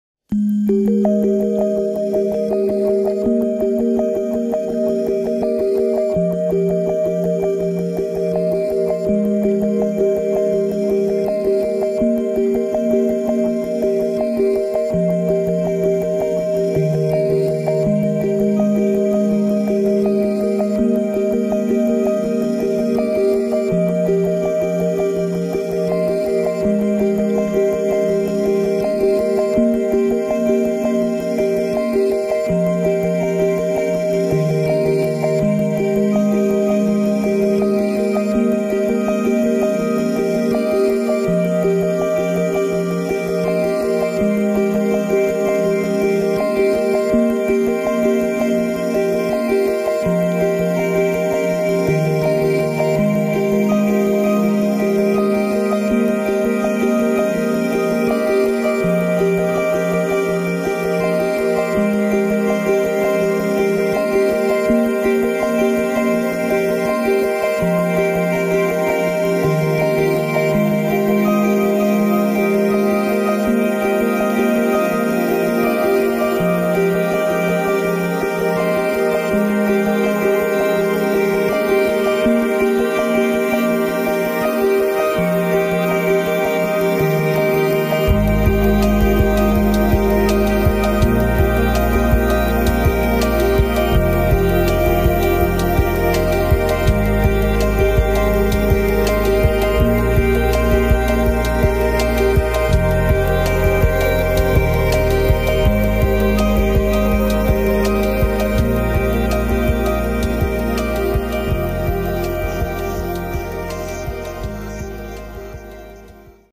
California Sea Lion Hunting Fish - Sea of Cortez - Mexico (alternate take)
California Sea Lion hunting
Gulf of California (sea of Cortez)
La Reina - La Paz - Baja California Sur - Mexico
[shot on a GoPro 4 Silver]